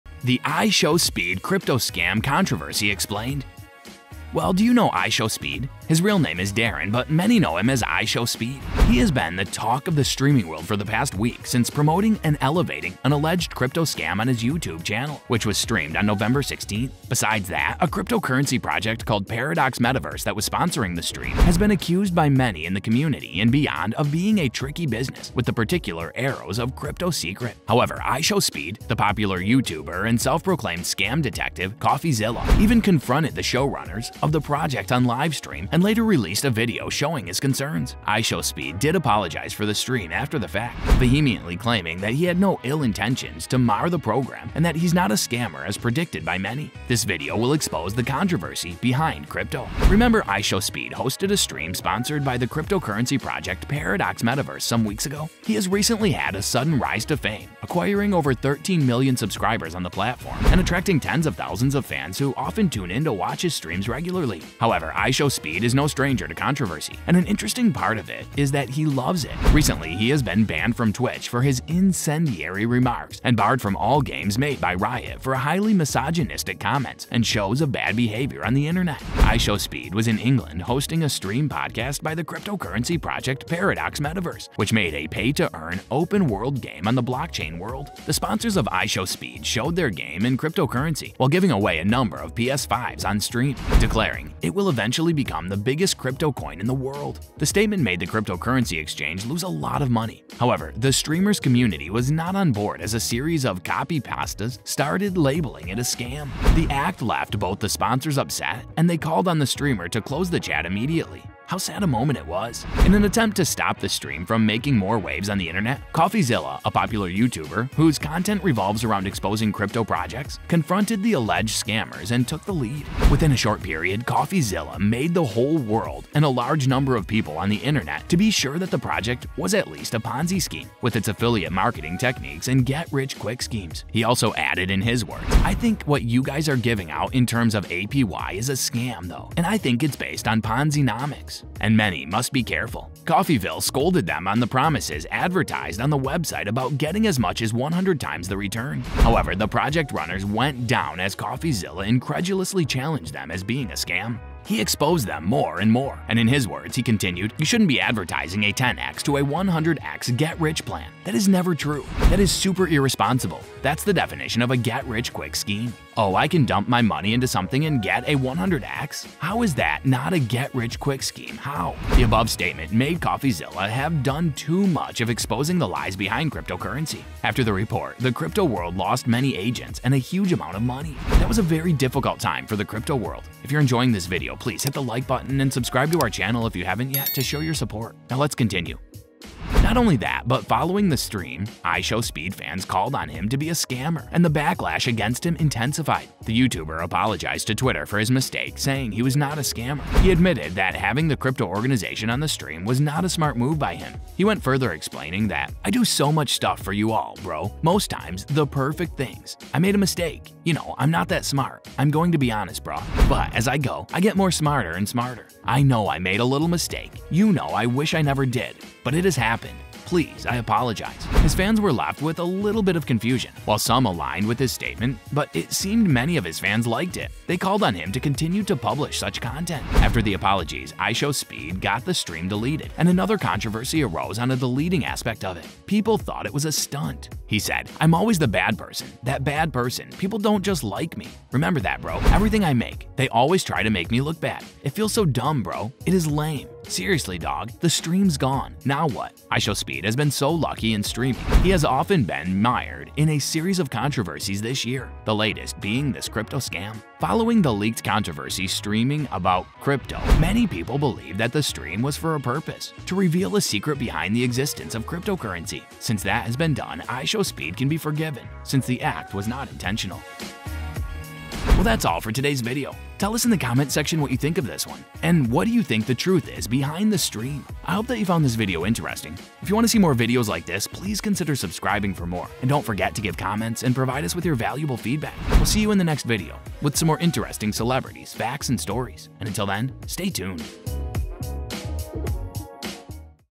IShowSpeed Crypto Scam Controversy EXPLAINED!
Hello there, and welcome to Luxfluent! Darren "IShowSpeed" has been the talk of the streaming world after promoting an alleged crypto scam on his YouTube stream on November 16. So in today's video, we will get to know about the scam.